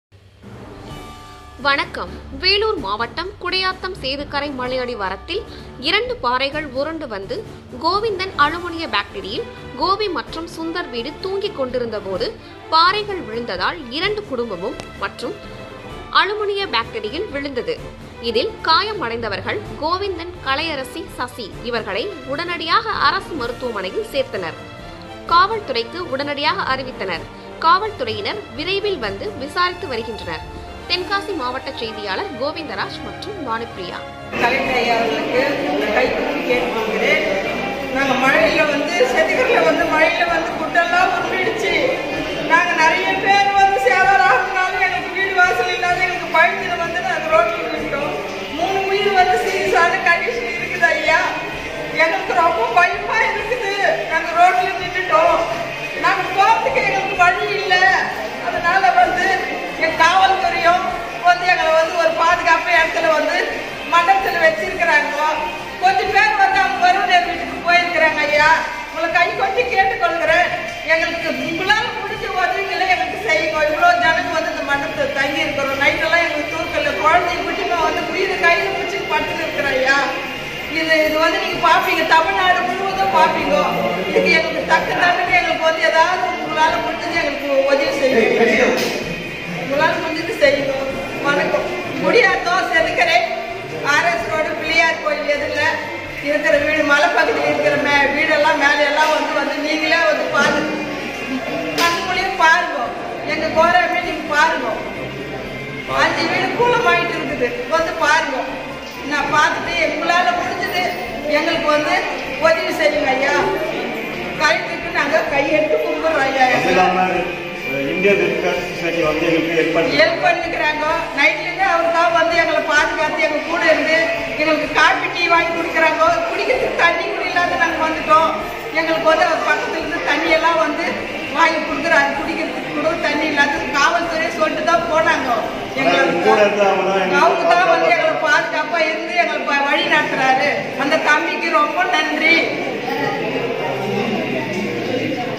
குடியாத்தம் மலையடிவாரத்தில் இரண்டு பாறைகள் உருண்டு விழுந்து வீடுகள் நாசம்@ROADSIDENEWSPRESS
வேலூர் மாவட்டம் குடியாத்தம் சேதுக்கரை மலையடிவாரத்தில் இரண்டு பாறைகள்.உருண்டு வந்து கோவிந்தன் அலுமினிய பேக்டரி கோபி மற்றும் சுந்தர் வீடு தூங்கிக்கொண்டு இருந்தபோது பாறைகள் விழுந்ததால் இரண்டு குடும்பமும் மற்றும் அலுமினிய பேக்டரியில் விழுந்தது இதில் காயம் அடைந்தவர்கள் கோவிந்தன் கலையரசி சசி இவர்களை உடனடியாக அரசு மருத்துவமனையில் சேர்த்தனர் காவல் துறைக்கு உடனடியாக அறிவித்தனர் காவல்துறையினர் விரைவில் வந்து விசாரித்து வருகின்றனர்